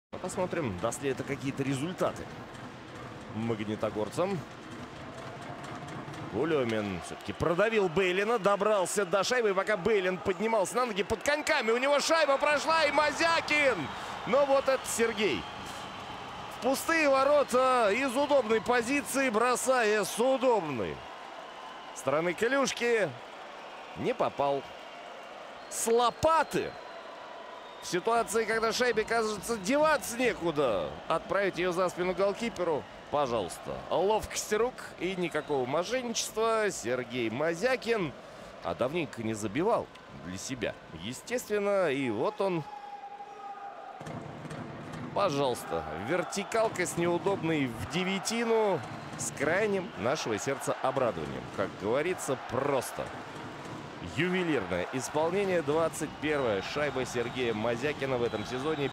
Мозякин сокращает отставание в счете
Сергей Мозякин переигрывает вратаря хитрым броском